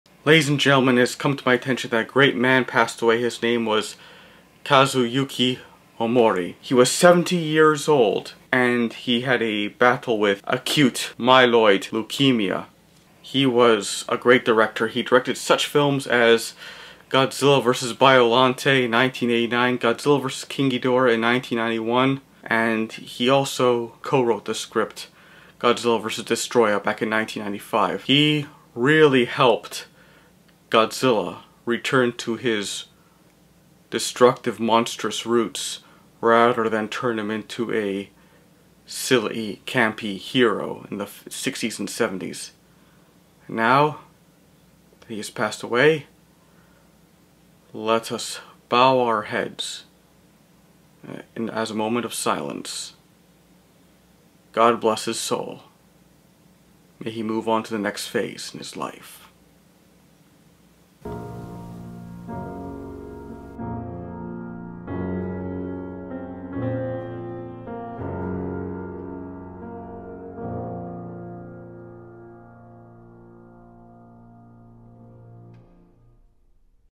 Goodbye Kazuki Omori.
As a kaiju fan, I know this is overdue, but it has come to my attention that Kazuki Omori has passed away.  He was the director of such films as GODZILLA VS. BIOLLANTE (1989) and GODZILLA VS. KING GHIDORAH (1991).  He also co-wrote the script for the film GODZILLA VS. DESTROYAH (1995).
May he rest in peace and God bless his soul.